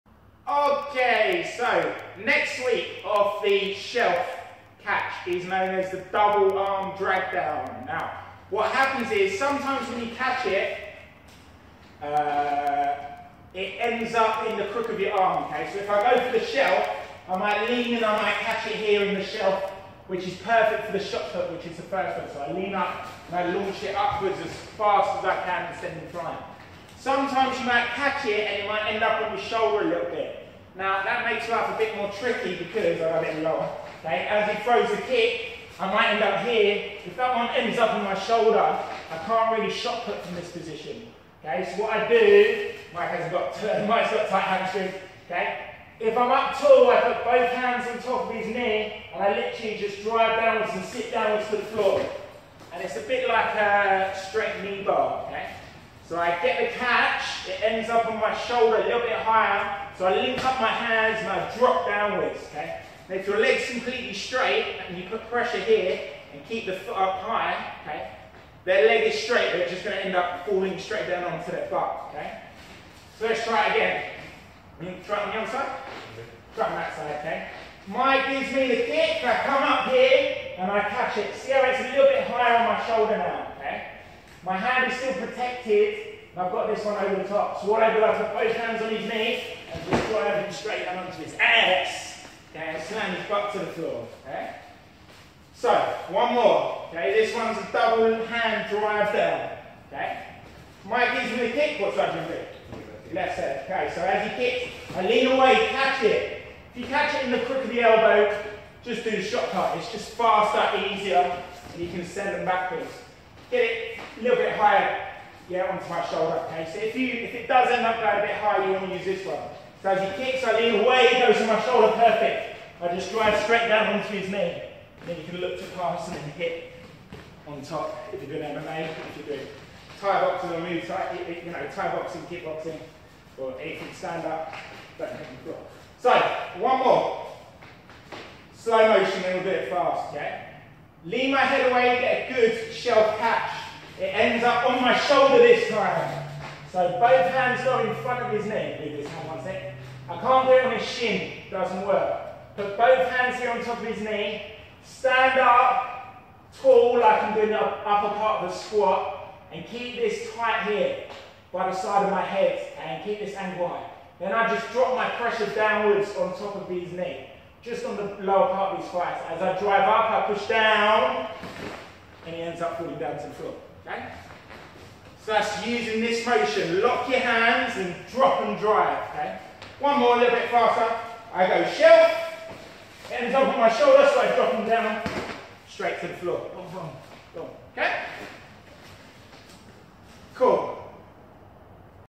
Double Arm Drag Down | Roundhouse Kick Catches & Takedowns
In this video I will teach you how to perform the double arm drag down takedown once you have caught your opponent's roundhouse kick using the shelf catch.